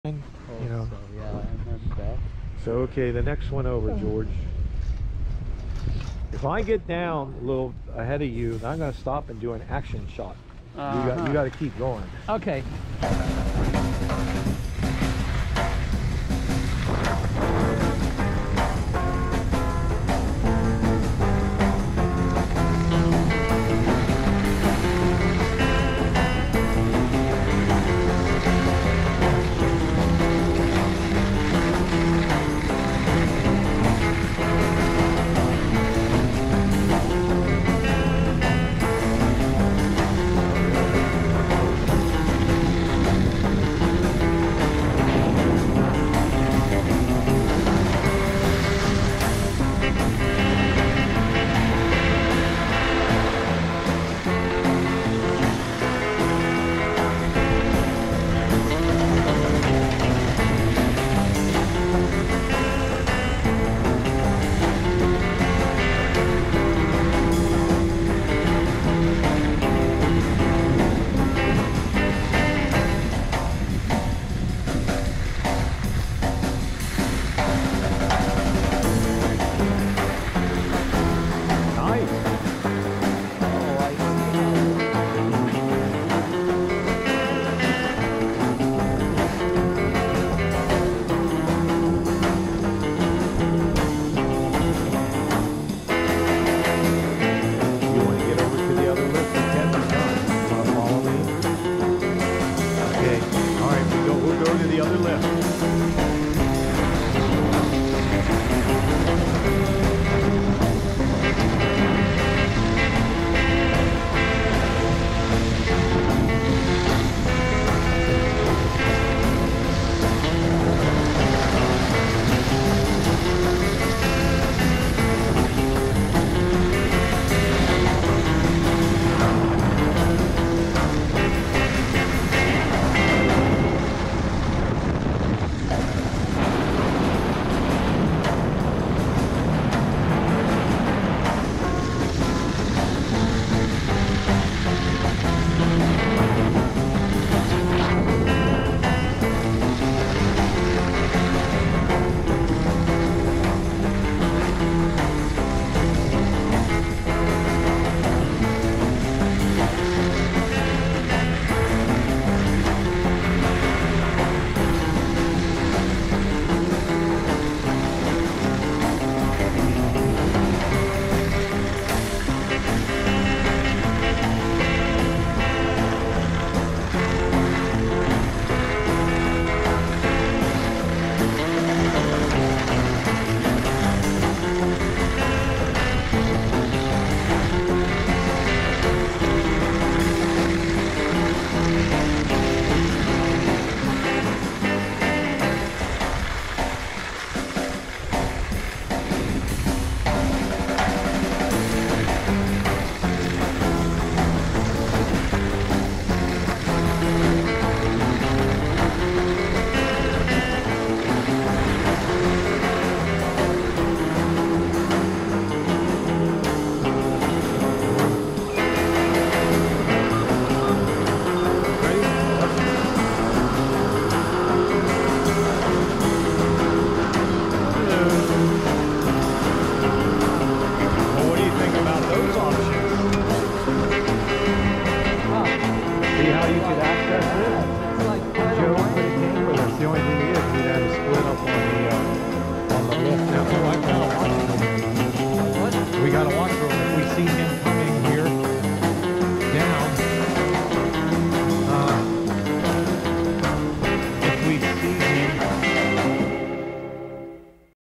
Ski Jack Frost ⛷️🏔️⛷️Snowflake to Easy RIder trails to Paradise lift 3/20/23 HD 4K w/ Telemetry
A nice (~32°f) March 20th day with excellent conditions at Jack Frost ski resort, Pennsylvania - Pocono Mountains on One Park.  Jackfrost Mountain in PA is a nice and well laid out and maintained ski mountain that's great for all levels of skiers.

Movavi Editor / GoPro Hero 9 Black / March 20th 2023 morning New Haven PA, USA